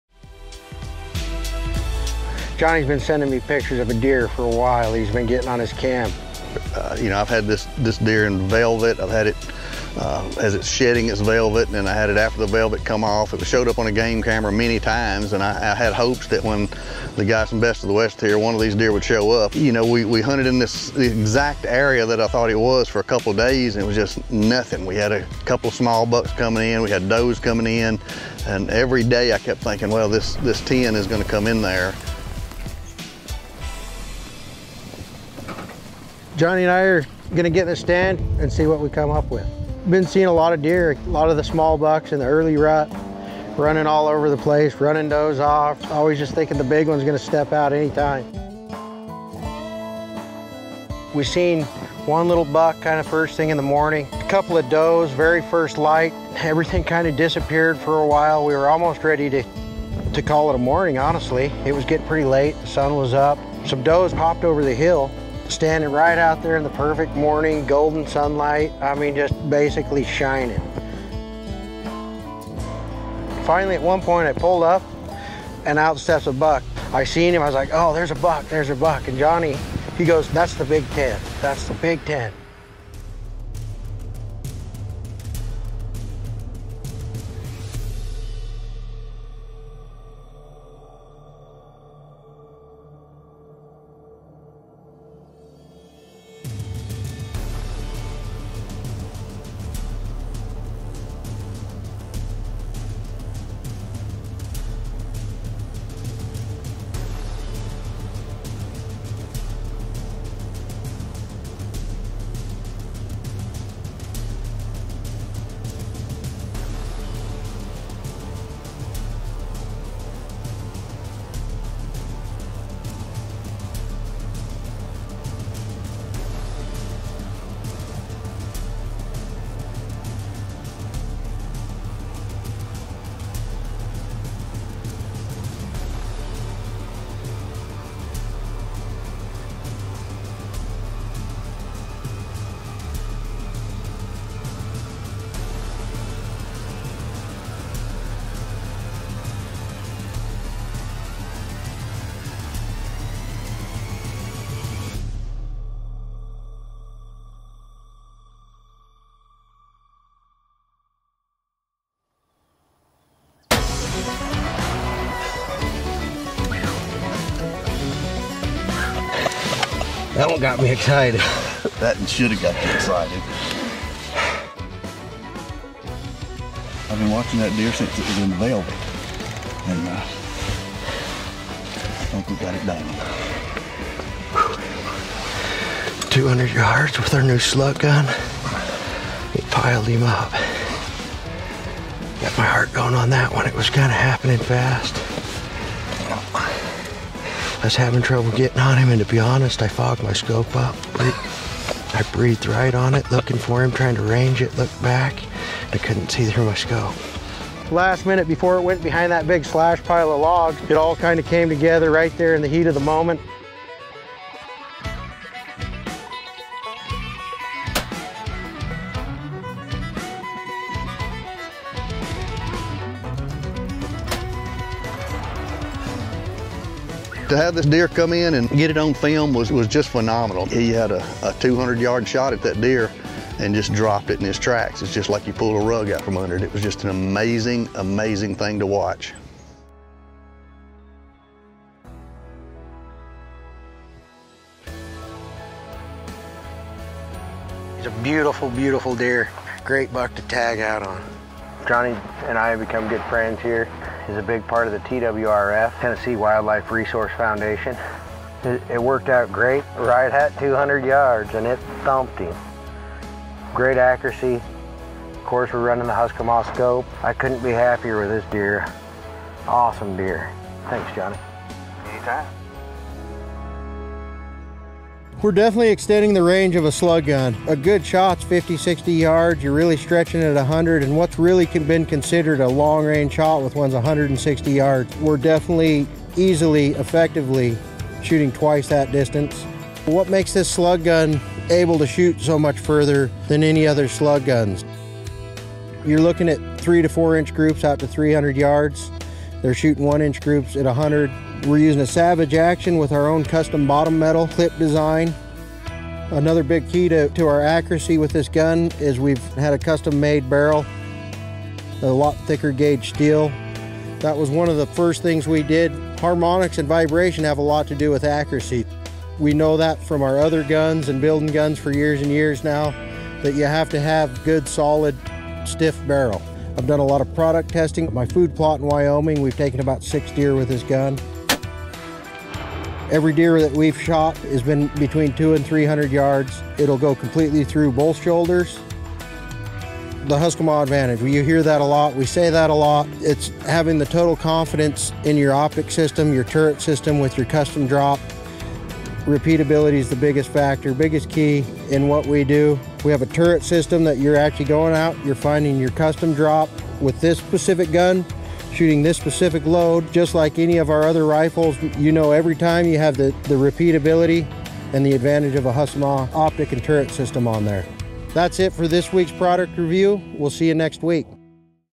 300 yard Slug Gun from Best of the West
Daryl Peterson puts the BotW Slug gun to the test at extreme ranges for whitetail in Tennessee.